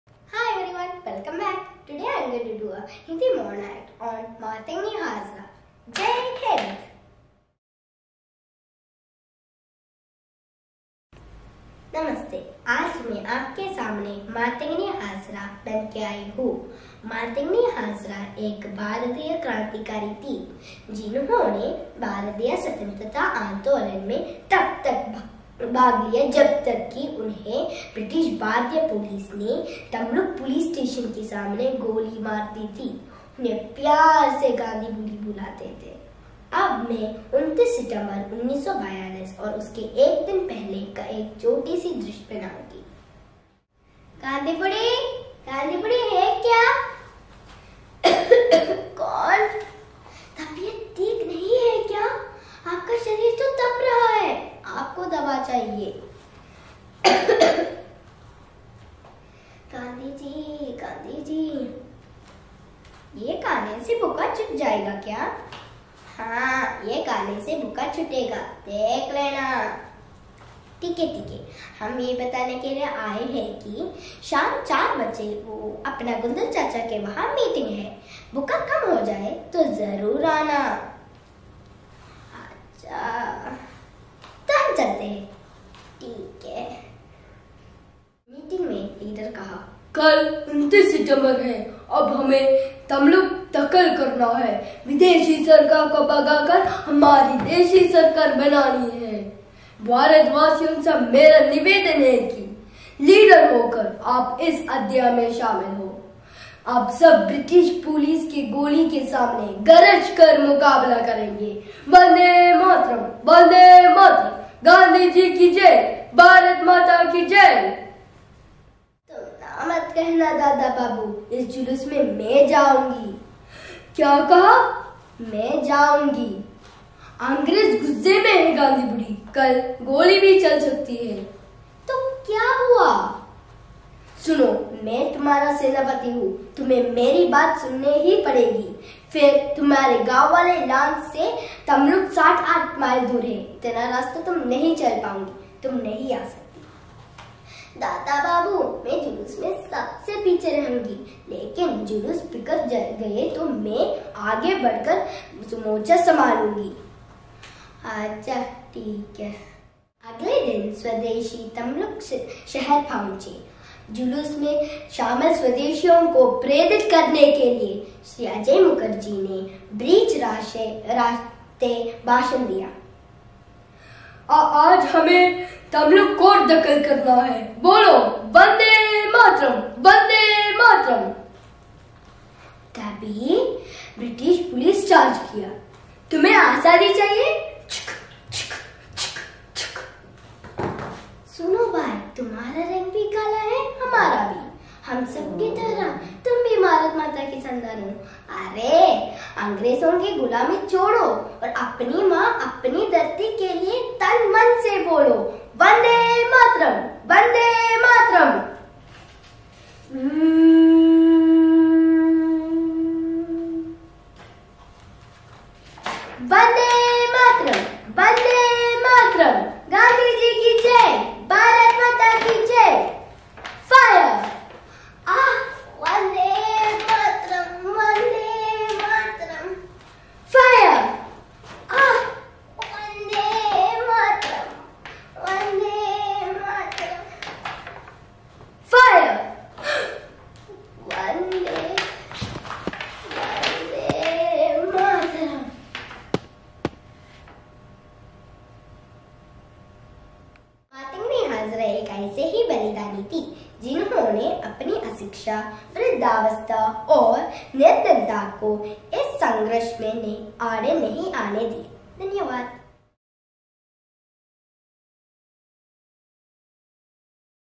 ROLE-PLAY/MONOACT ON FREEDOM FIGHTERS| MATANGINI HAZRA| UNSUNG INDIANFREEDOMFIGHTER|COMPETITION IDEA
This competition was not a live event , we needed to submit video and editing was allowed.